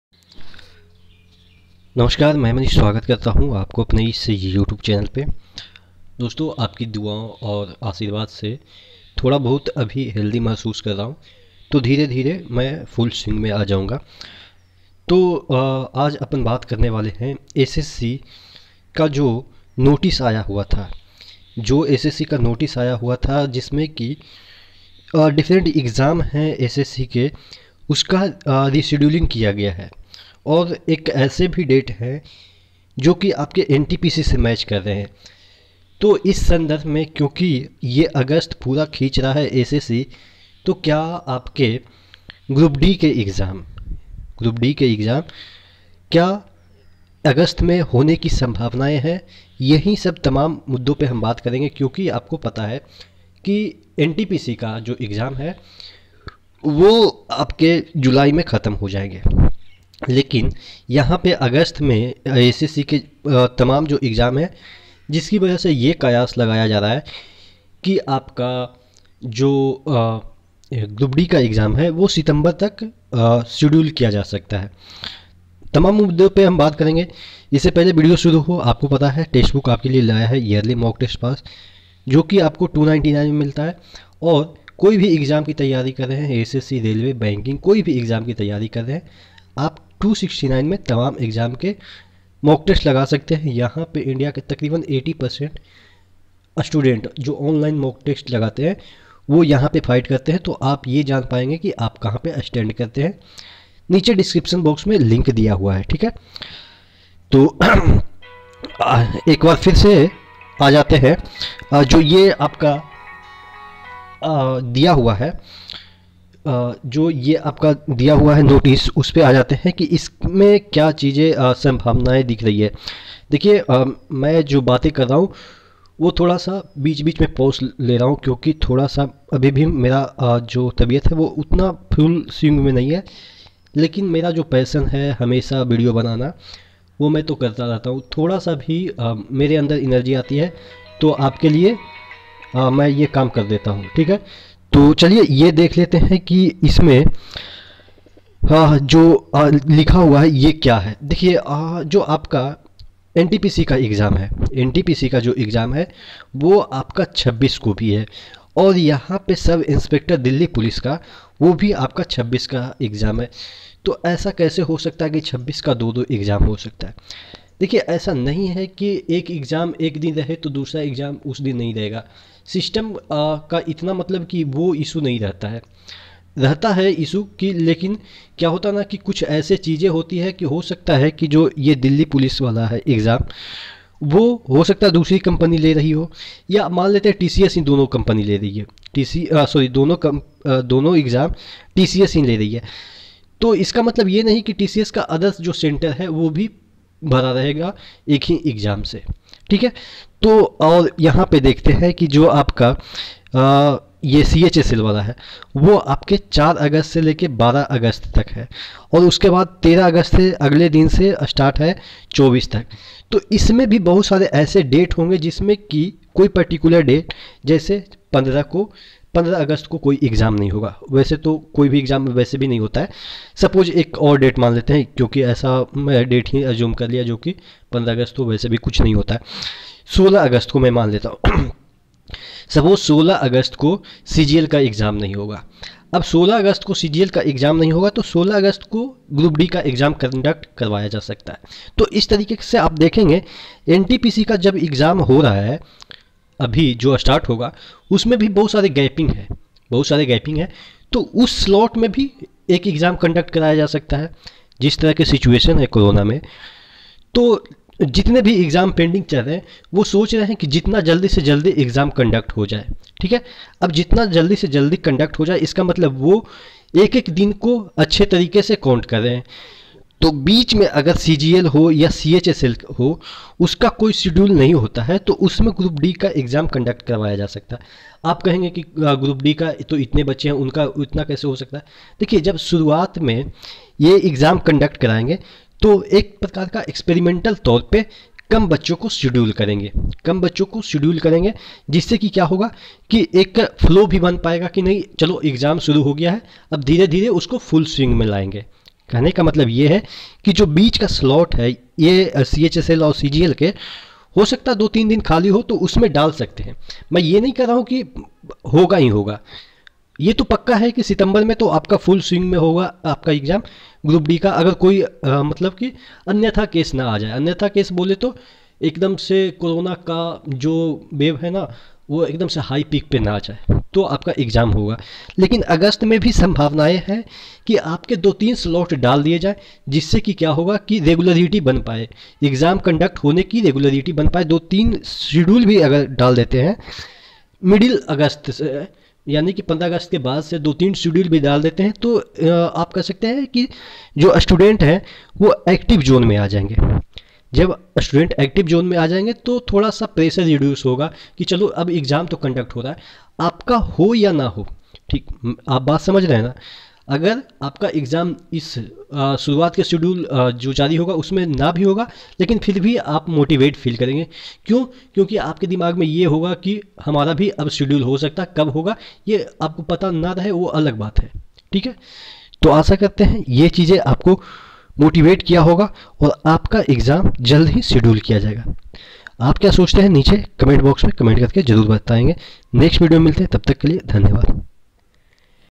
rrc group d exam date 2021/rrc group d exam schedule confusion/rrc group d exam may be in August
Mera coupon “ERMANISH” lagaiye aur ispe 30 rupye ka extra discount paayein